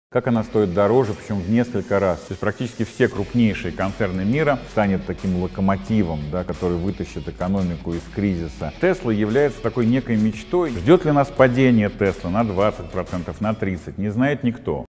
Анонс ролика 23 | Феномен компании Tesla
Представляем цикл полезных роликов для начинающих инвесторов от Михаила Ханова (ООО "Алго Капитал").
В этом видео Михаил рассказывает о феномене компании Tesla.
ООО "Алго Капитал"
Лицензия ЦБ РФ №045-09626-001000 от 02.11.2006 года